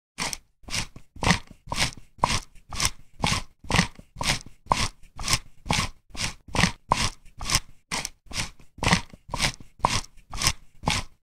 Handheld pepper mill grinding pepper - Sound effects
The sounds of a handheld pepper mill grinding pepper.